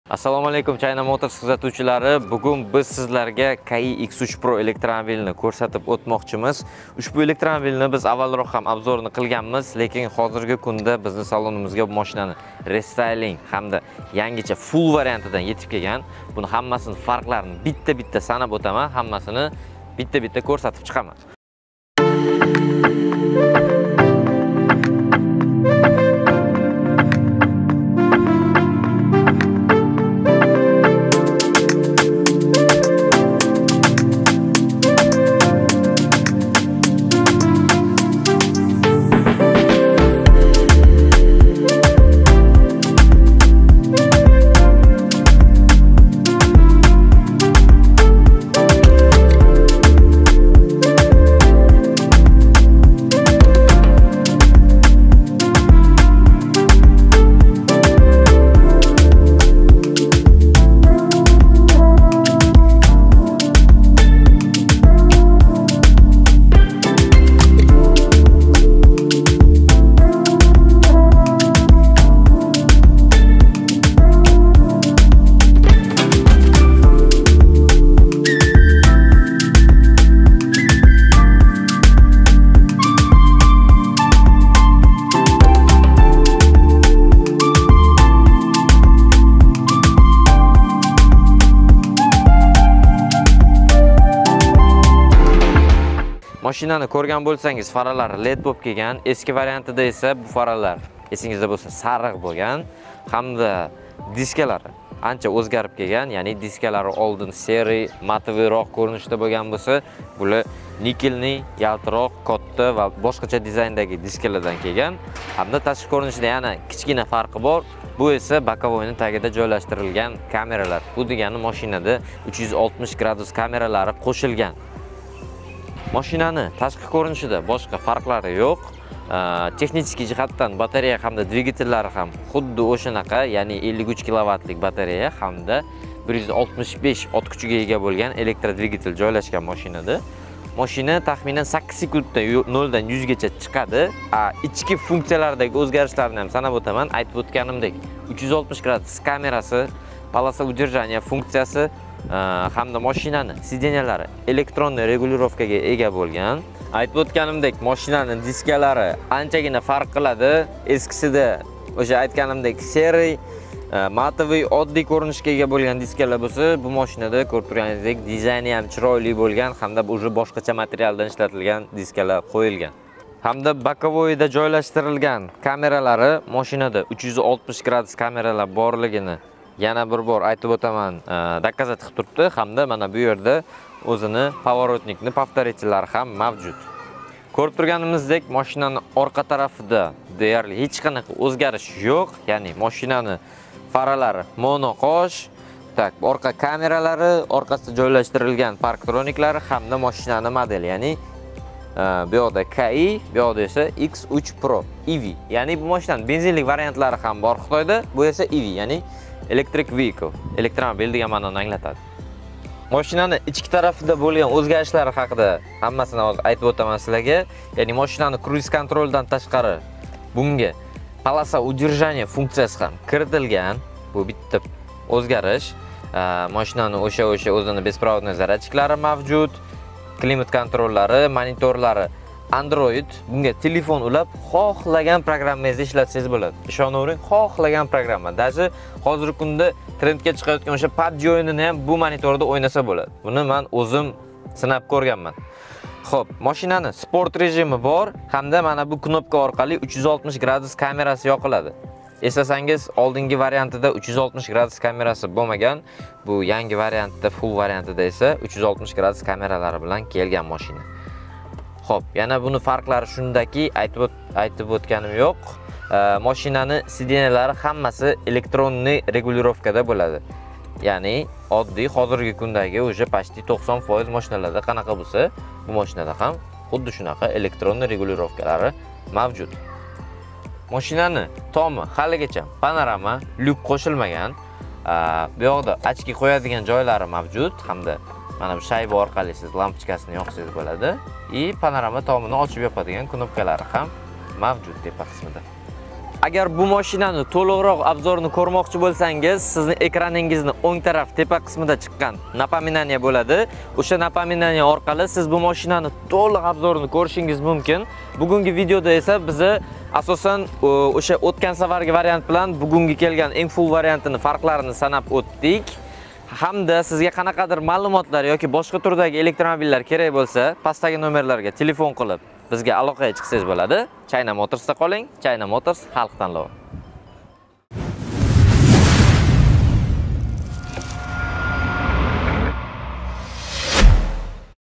KAIYI X3 PRO Электромобили.15.000 сўмга 450 км. Бу энди нимаси!?.
Буюртма бериш ёки қўшимча саволлар учун қуйидаги рақамларга мурожаат қилинг